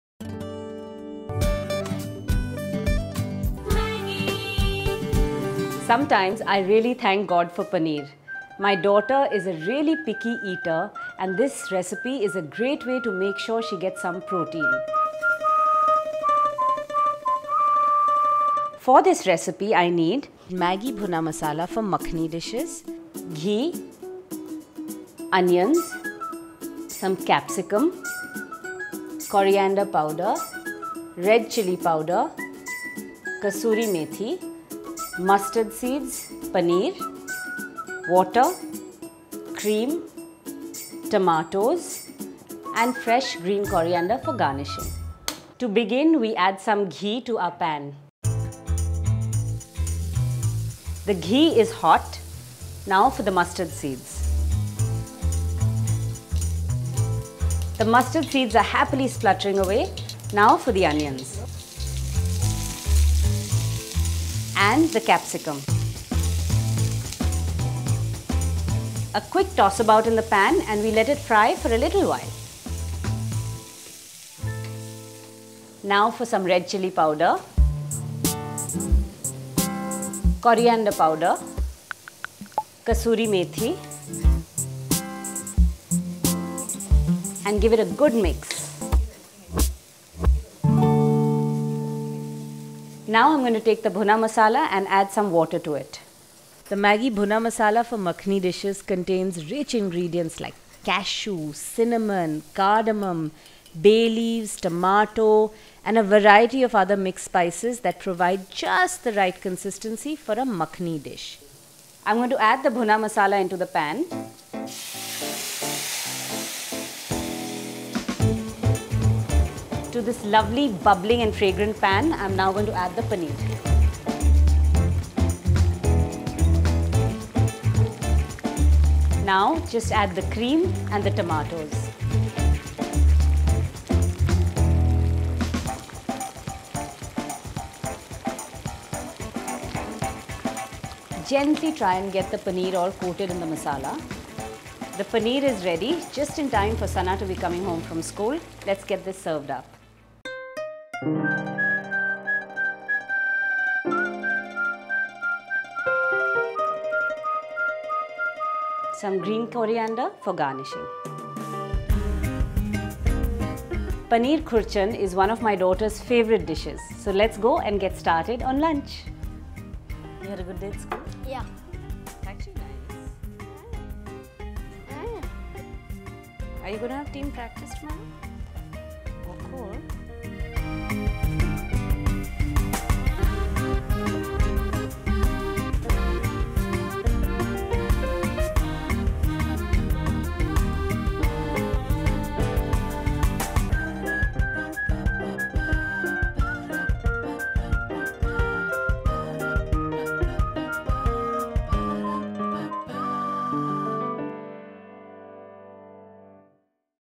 Paneer Khurchan Recipe| MAGGI Creative Kitchen
Paneer Khurchan - Yummy Paneer cooked in tomato gravy with a veggie twist. MAGGI Creative Kitchen brings to you an easy and simple way to cook restaurant style Paneer Khurchan at home.